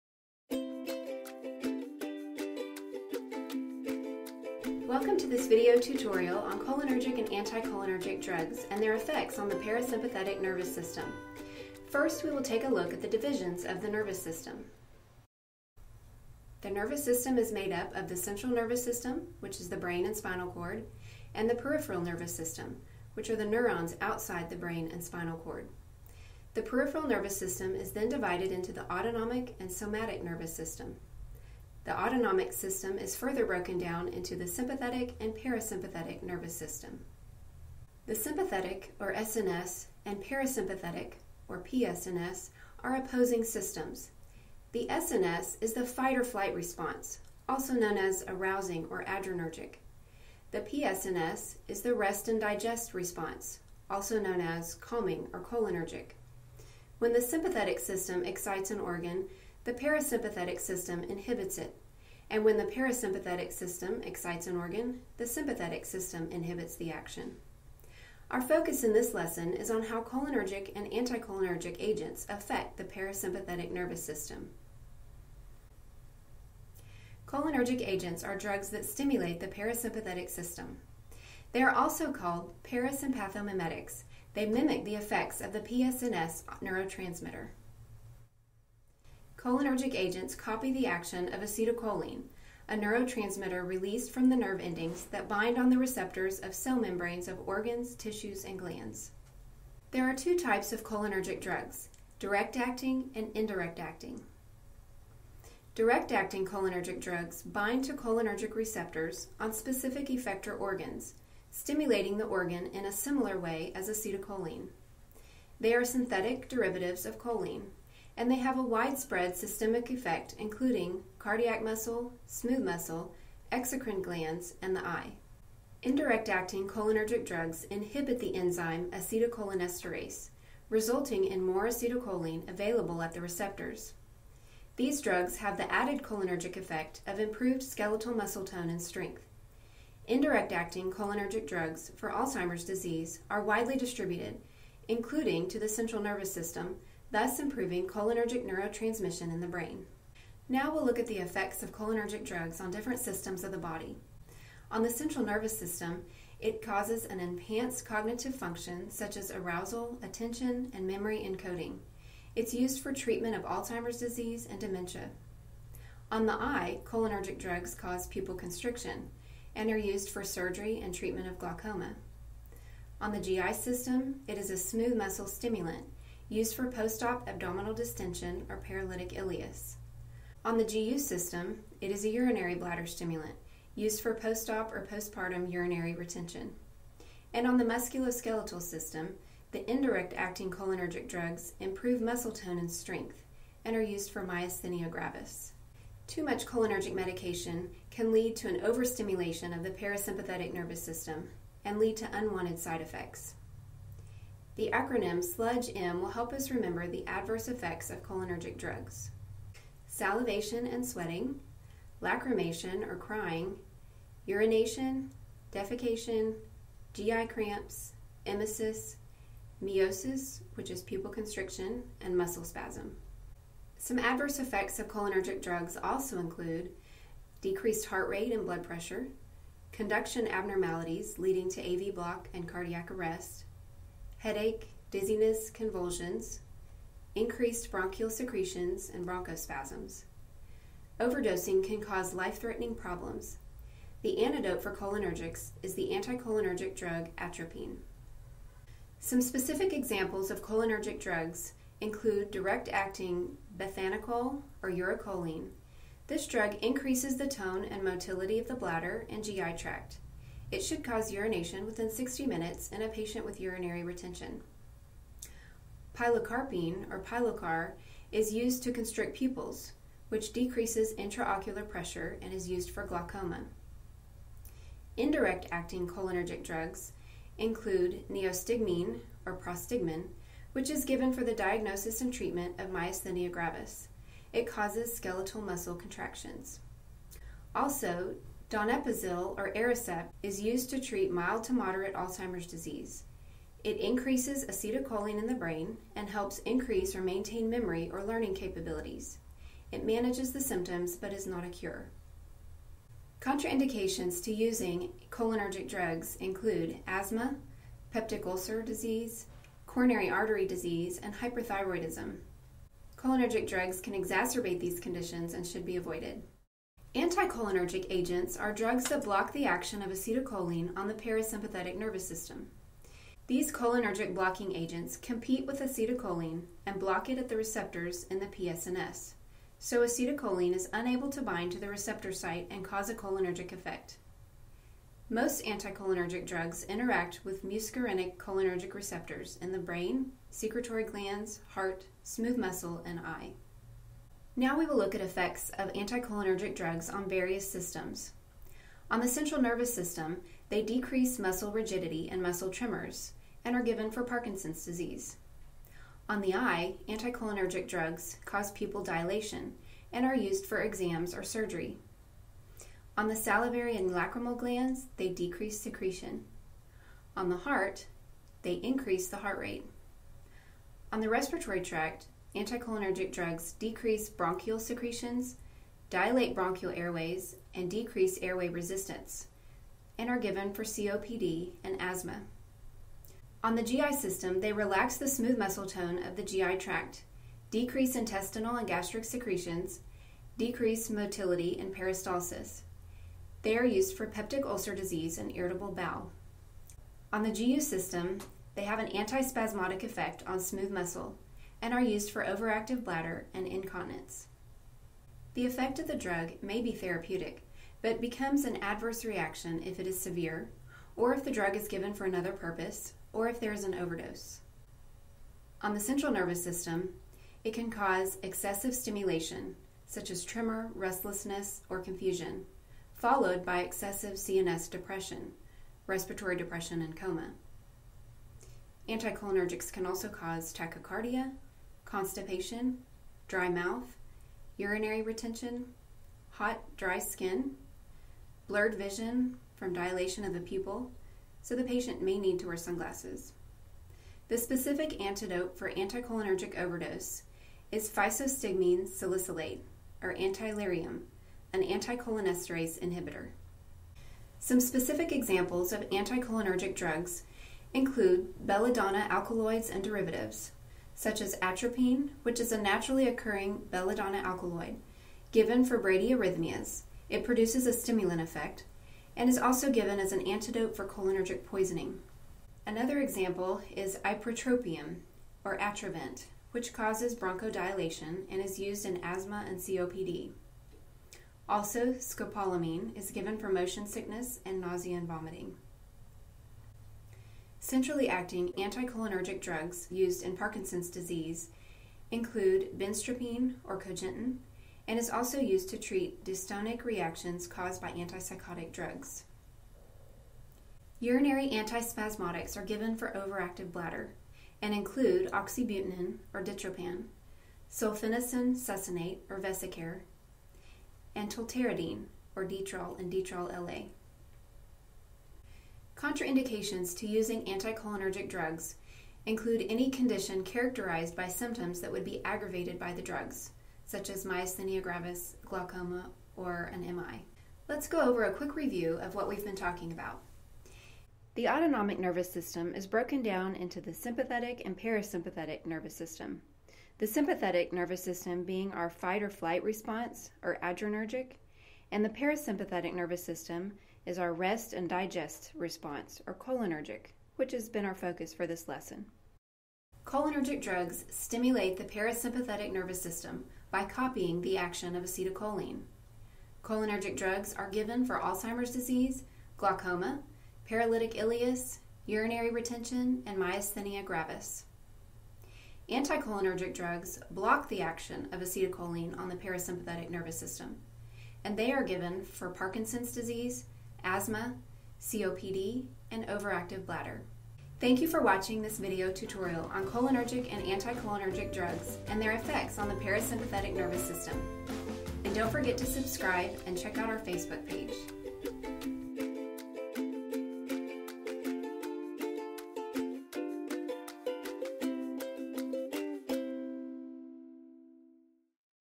NCLEX RN Review: Cholinergic (PNS) and Anticholinergic (SNS) Drugs
Know the difference and when to use them.  This video review breaks down all that you need to know about cholinergic drugs.

STAY IN TOUCH!